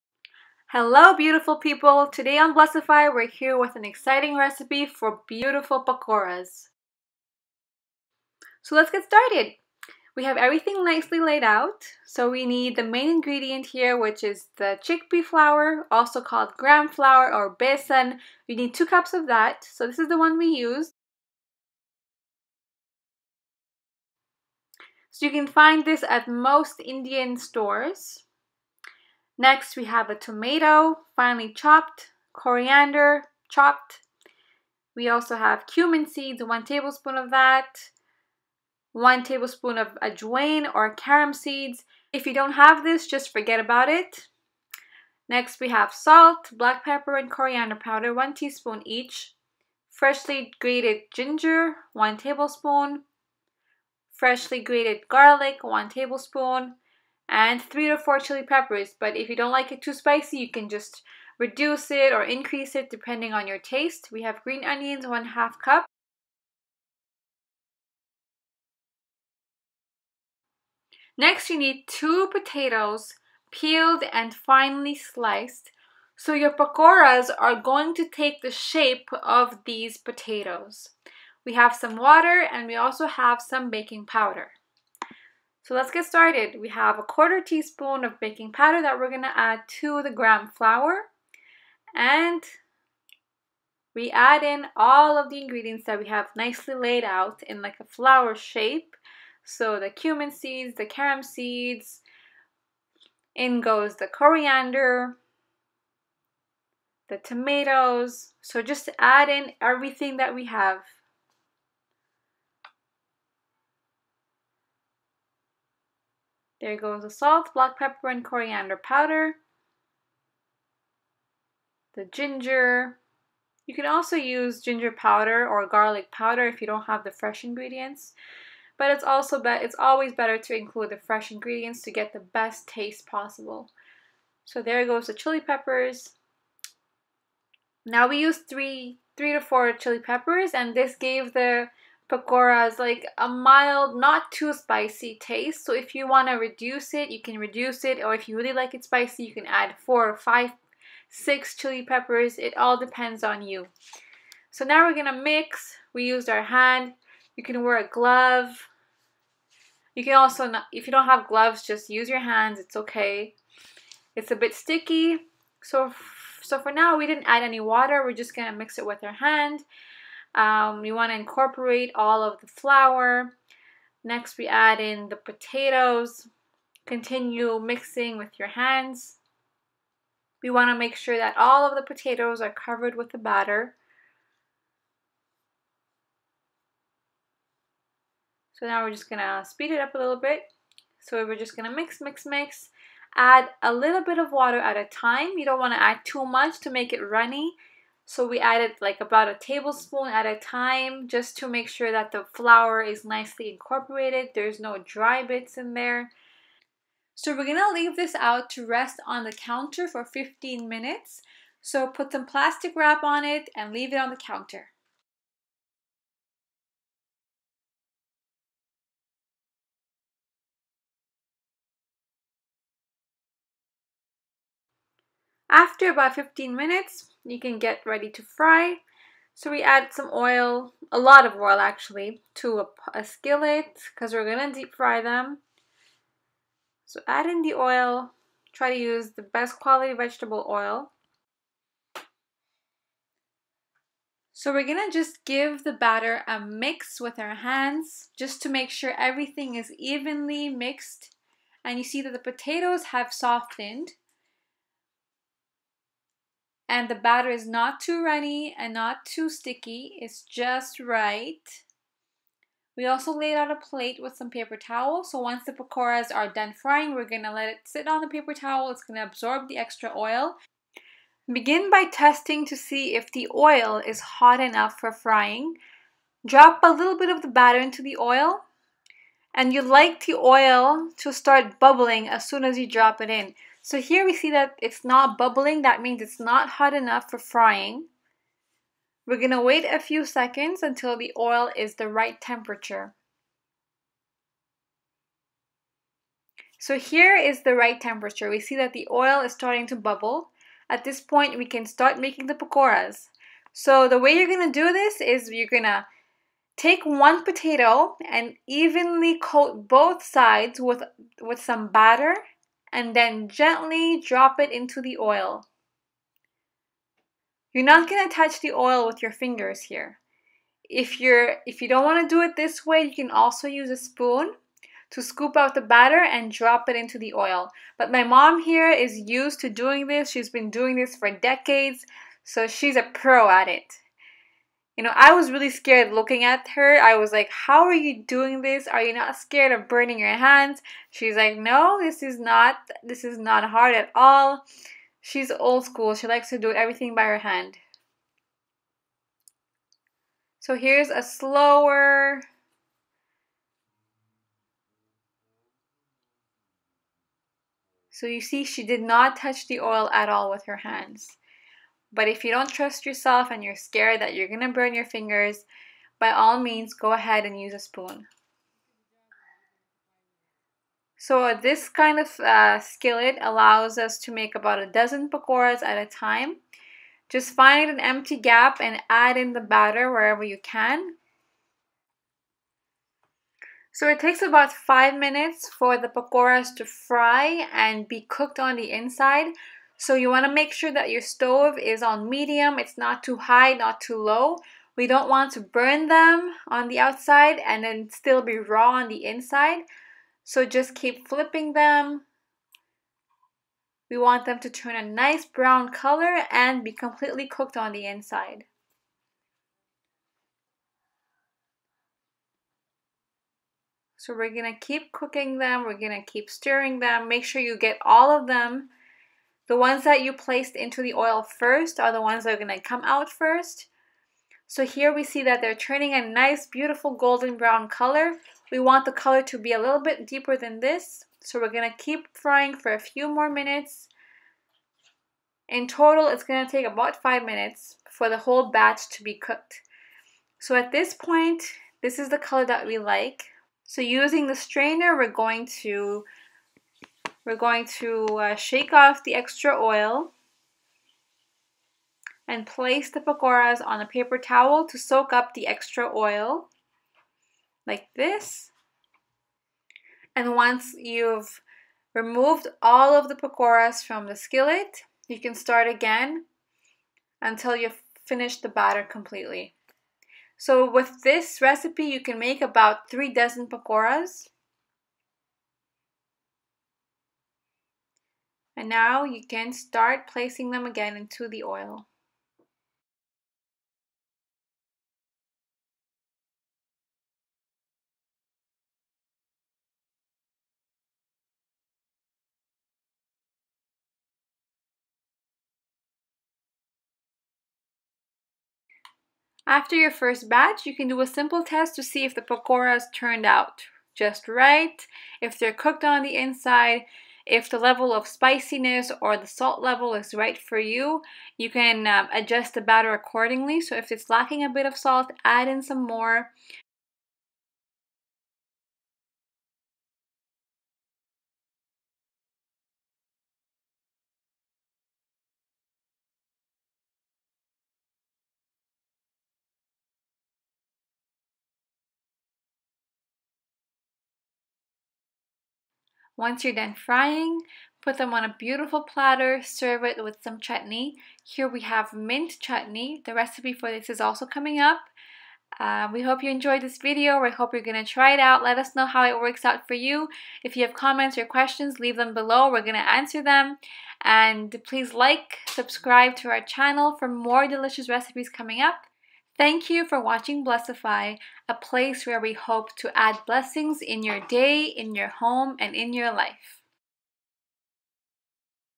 EASY potato PAKORA Recipe [Gluten-free, Vegan] پکوڑا
FOOL-PROOF pakora recipe using simple ingredients. Crispy on the outside and soft on the inside.

Ingredients:
2 cups chick pea flour (gram flour/besan)
1/4 tsp baking powder
1/2 cup chopped coriander
1 medium tomato chopped
1/2 cup green onions chopped
1 tbs cumin seeds
1 tbs carom seeds (ajwain)
1 tsp salt
1 tsp black pepper powder
1 tsp coriander powder
1 tbs grated garlic
1 tbs grated ginger
2 potatoes finely sliced